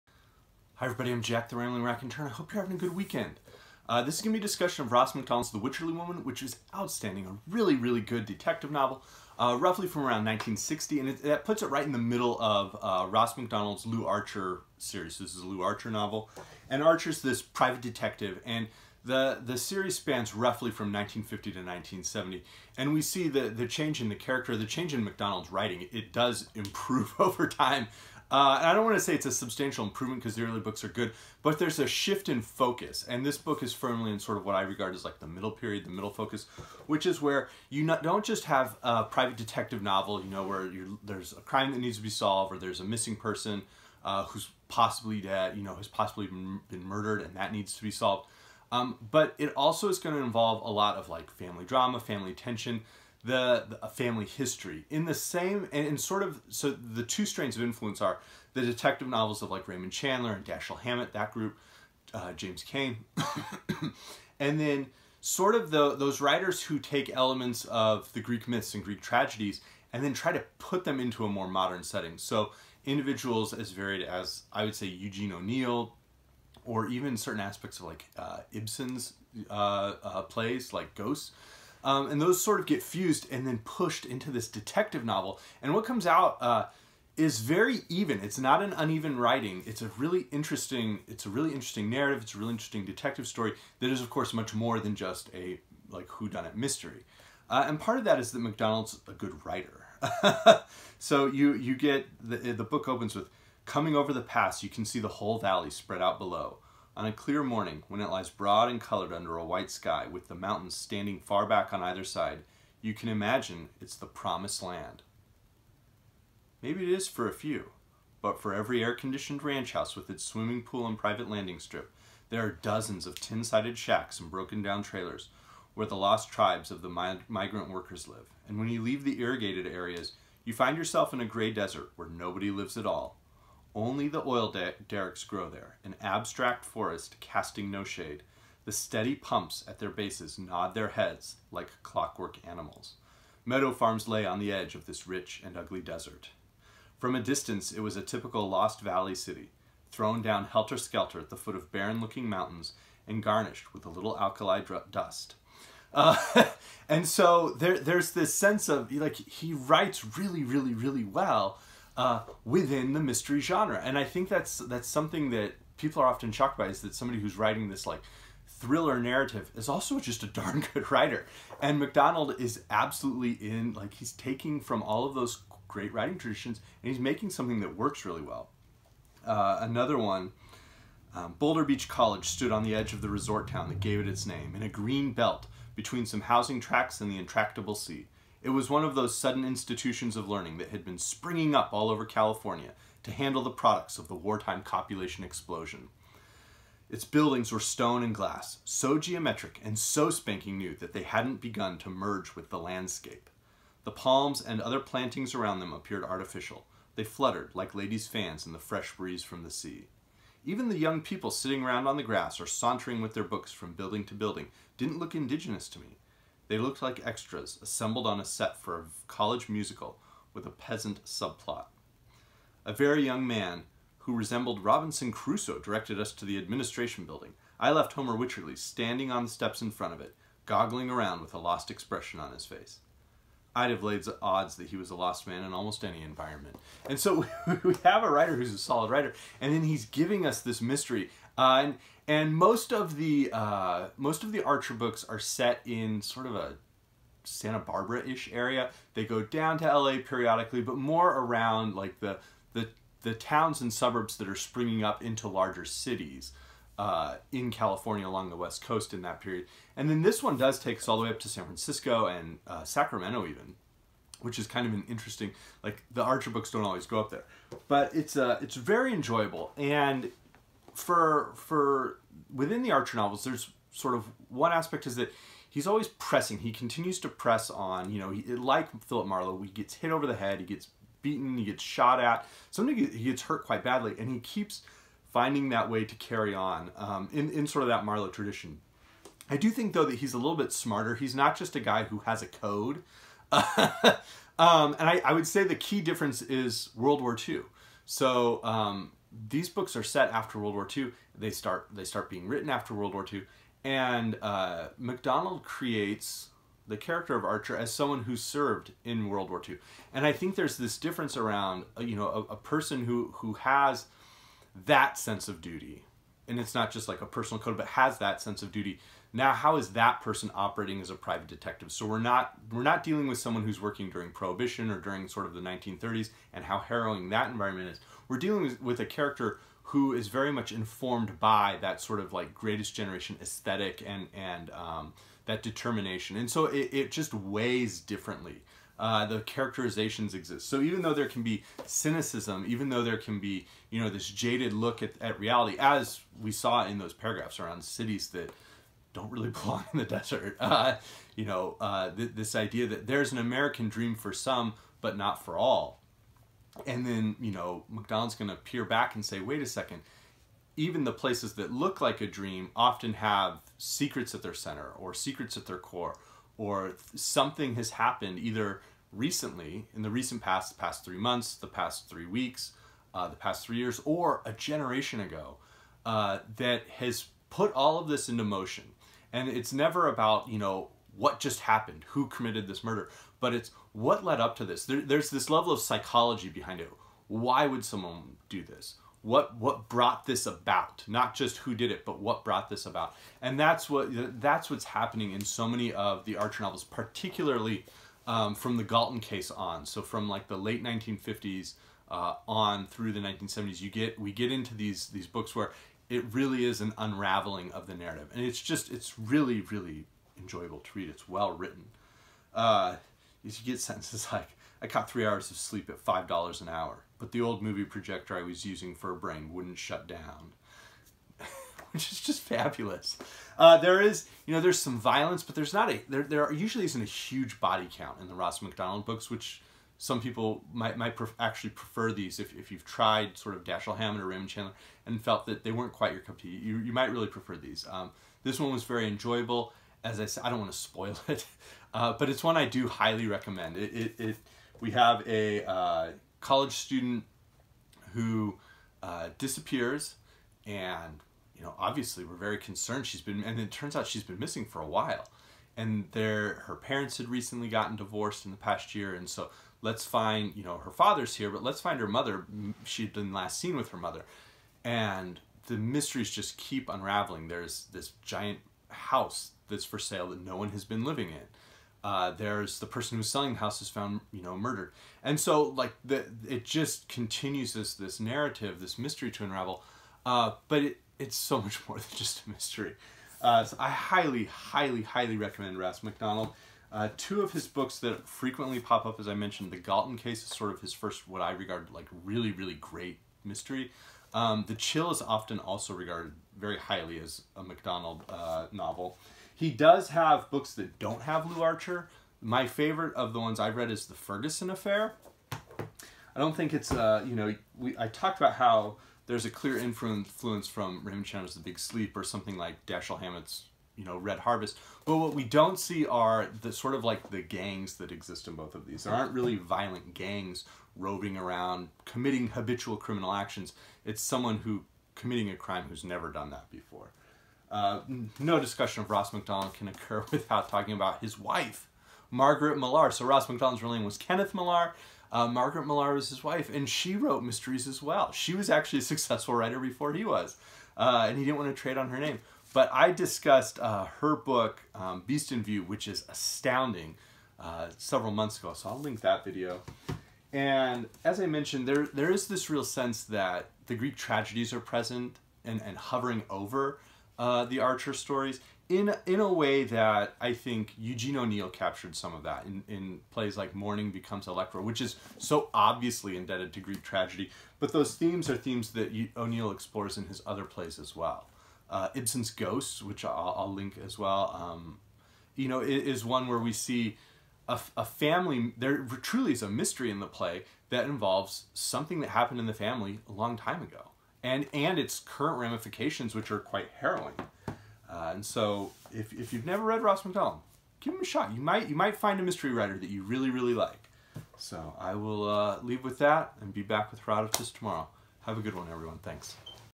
The Wycherly Woman by Ross MacDonald (a detective novel informed by Greek tragedy!)
A discussion of MacDonald’s great mystery with some thoughts on what he introduced in detective fiction.

Recommended if you like:
The Galton Case by Ross MacDonald
The Chill by Ross MacDonald
The Ferguson Affair by Ross MacDonald (not an Archer mystery)
Greek tragedies: Oedipus Tyrannus by Sophocles, Electra by Euripides
Mourning Becomes Electra by Eugene O’Neill
The Maltese Falcon by Dashiell Hammett
The Big Sleep by Raymond Chandler